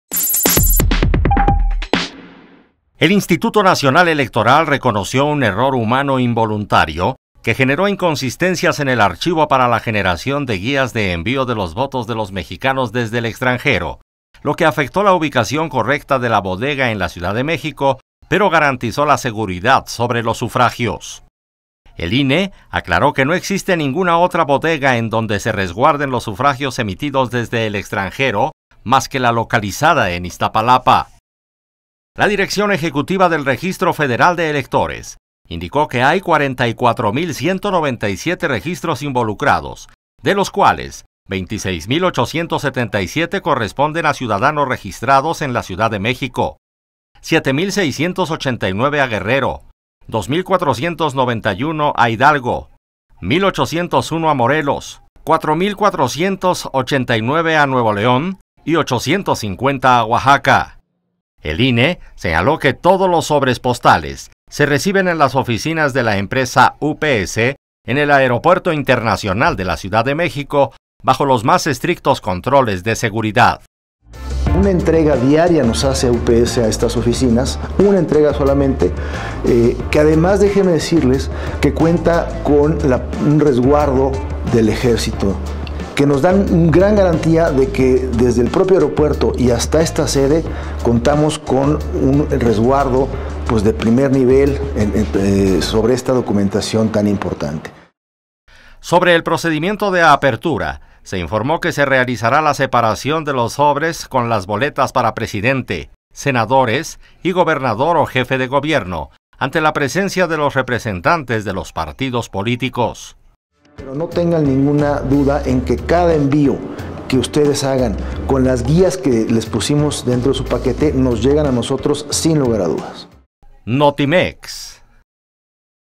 Pese a inconsistencia, INE garantiza seguridad sobre votos desde el exterior
El “error humano involuntario” afectó la ubicación correcta de la bodega del INE en la Ciudad de México.